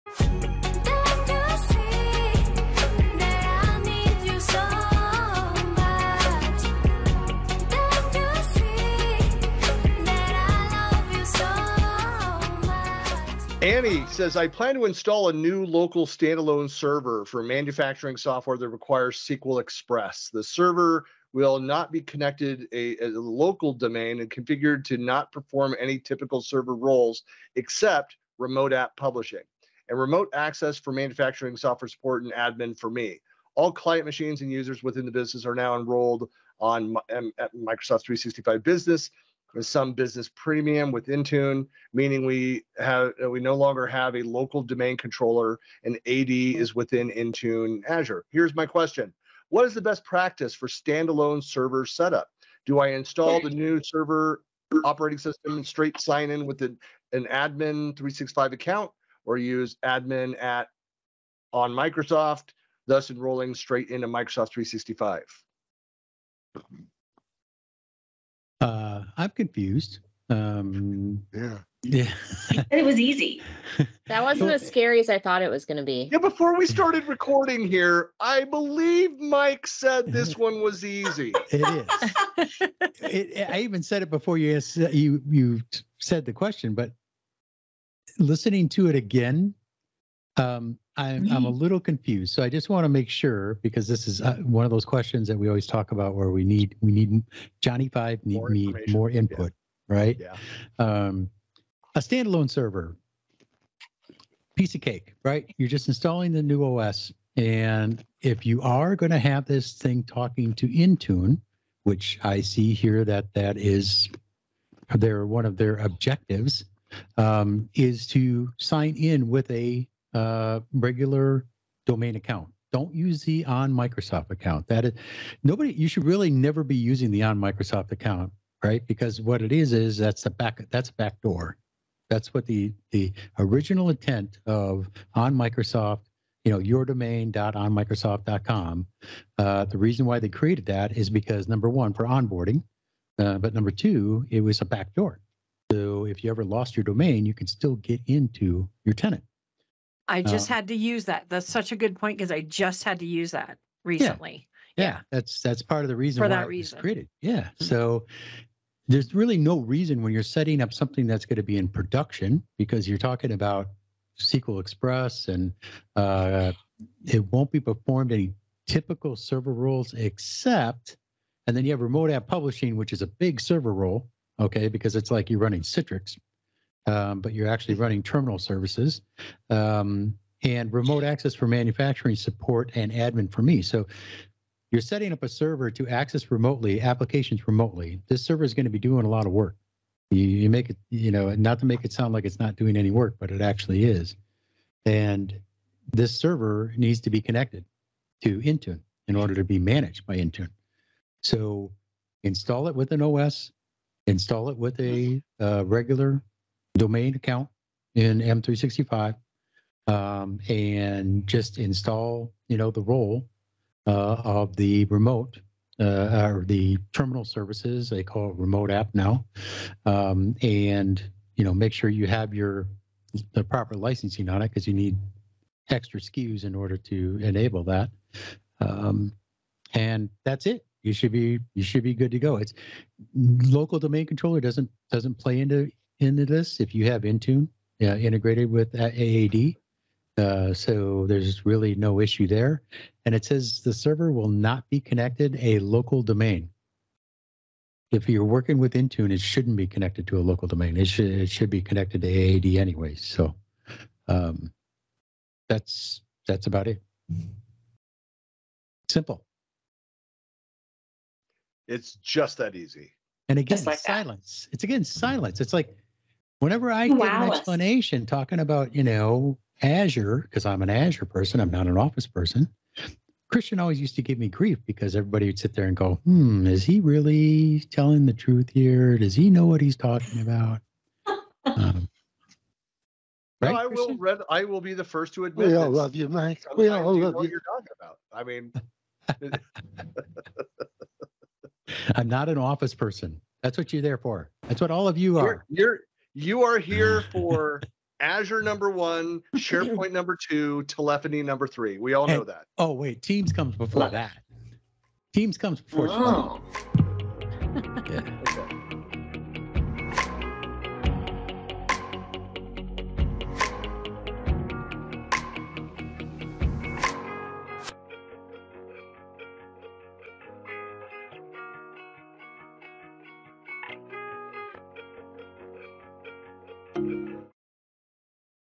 M365AMA What are the best practices for setting up a standalone server?
In this episode, the M365AMA panel discusses the following community question

“I plan to install a new local standalone server, for manufacturing software that requires SQL express. The server will not be connected a ‘local’ domain and configured to not perform any typical server roles except remoteApp publishing. And remote access for manufacturing software Support and Admin for me. All client machines / users within the business are now enrolled onto MS365 business, and some business premium (Intune), meaning we have no longer have a local domain controller and AD is within Intune / Azure. Here is my question…. What is best practice for standalone server setup? Do I install the new server OS and straight sign in with a admin365 account.. or use account? thus enrolling straight into MS365?"

Some relevant notes/links shared by the team:

-- Ronald Caldwell on LiquidWeb walks you through How to Set Up a Dedicated Server
-- For those interested in some coursework, Udemy has an offer: Master How to Install a Windows Server for a Small Business